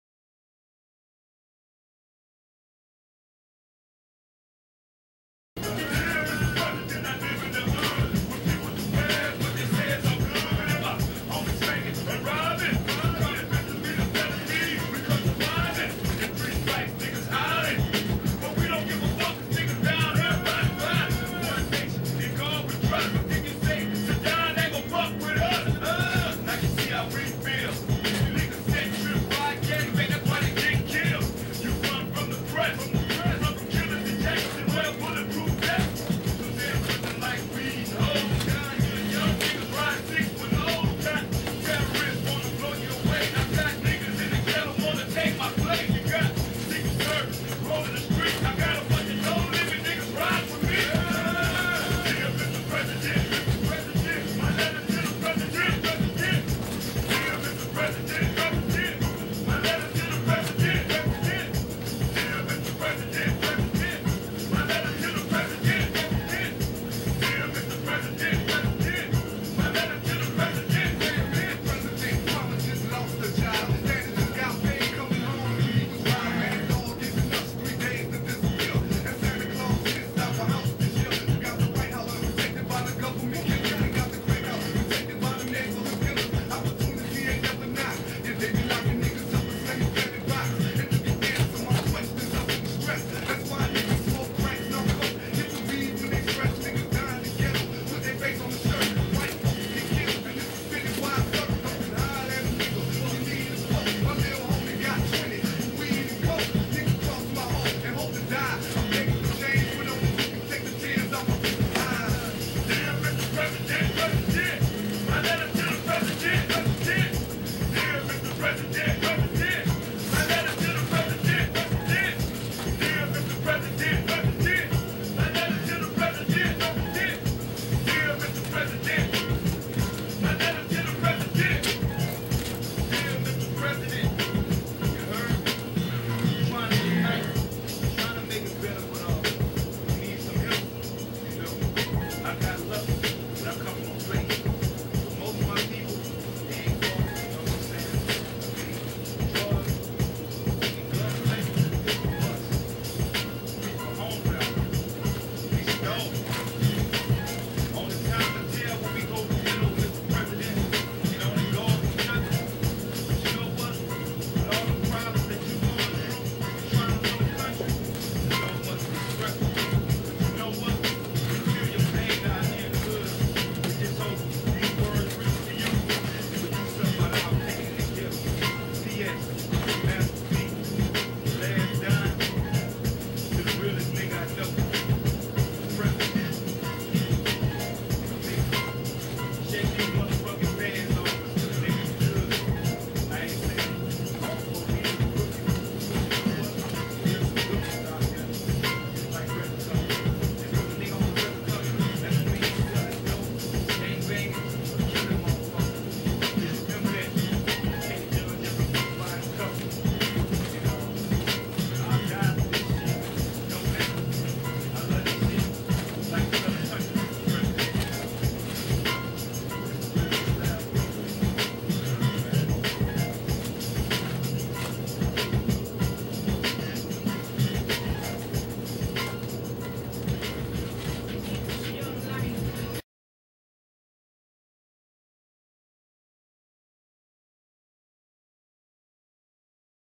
+T+R+U+T+H+U+R+T+Z+MAZTER+P+D3AR+MR+PR3ZID3NT+ANTICHRIST+OBAMA+GODZ+GOT+SWAGG+OOO+W33+
Y3Z ITZ TRU3 OBAMA IZ TH3 ANTICHRIST !!! OOO W33 T.R.U.T.H.U.R.T.Z.
IF J3SUS WAZ A DARK-SKINNED-MAN-WITH-HAIR-LIKE-WOOL- !
WHAT COLOR MAN DO U THINK THE DEVIL ILL SEND 2 TRY AND DUPLICATE THAT ??? OOO WEE ITZ TIME 2WAKE UP AMERIKKKA WE R AT WAR !!!